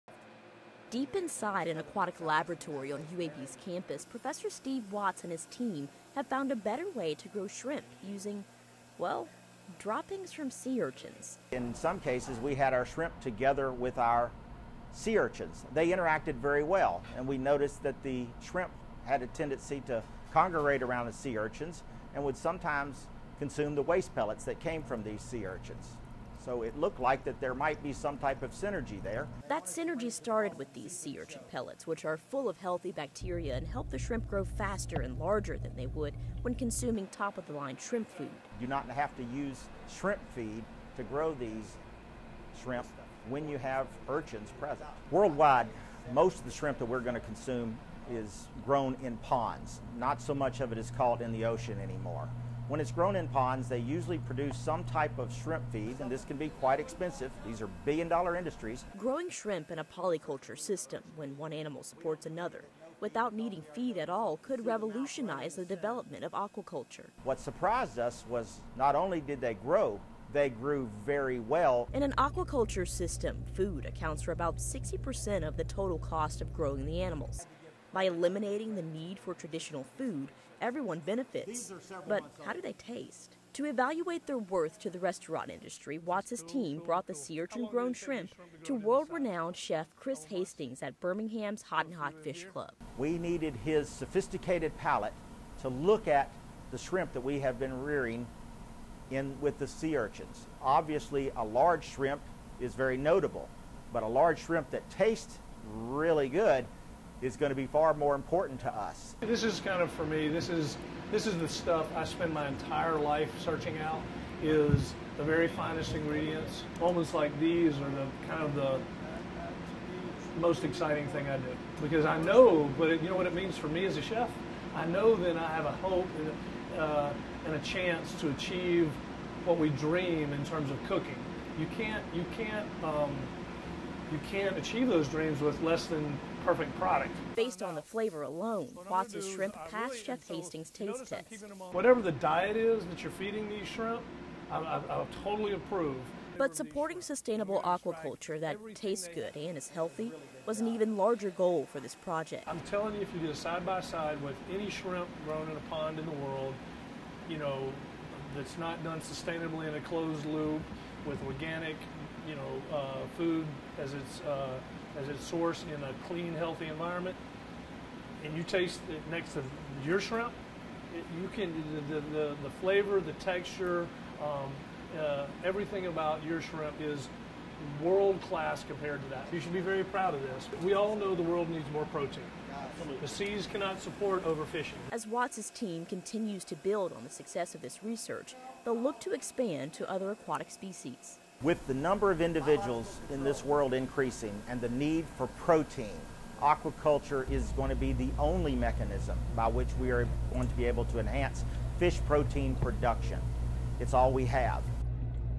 Urchin droppings grow super sized shrimp
Researchers at the University of Alabama at Birmingham appear to have found a better way to grow shrimp that is also less expensive, and the new process could hold the key to unlocking future breakthroughs in environmental science, business and medicine.